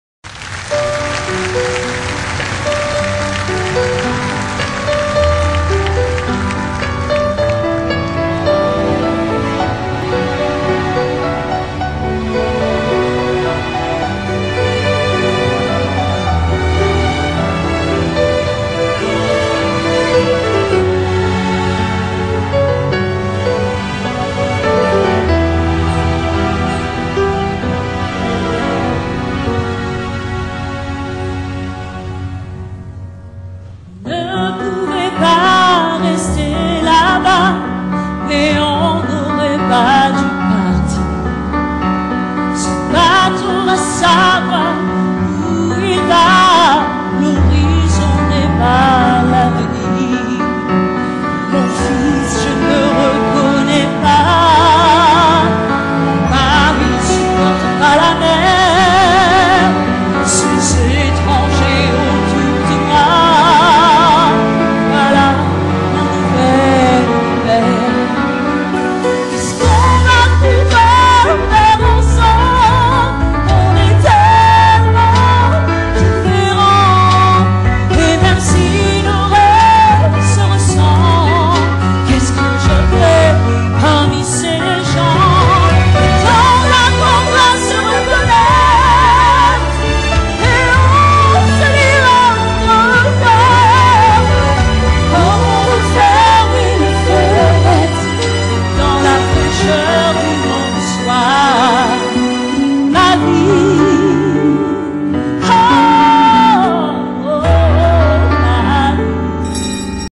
On ne pouvait pas rester là-bas
Marie Lacombe regrette d'avoir quitté l'Algérie.